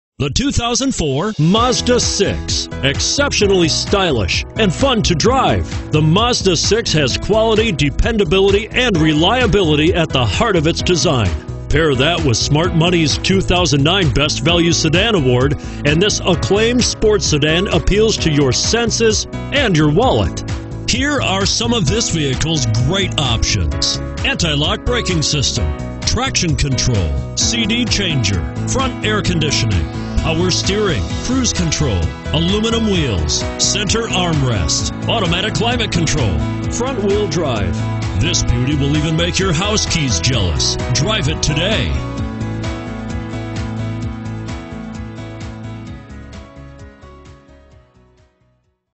2004 Mazda MAZDA6 Minneapolis St. Cloud & Monticello MN B14-99A
2004 Mazda MAZDA6 s

Looking for the right car? Today could be your lucky day. With 192344 miles, this Onyx Black, 2004 Mazda MAZDA6 equipped with Automatic transmission could be yours. Request more information and set up a test drive right away.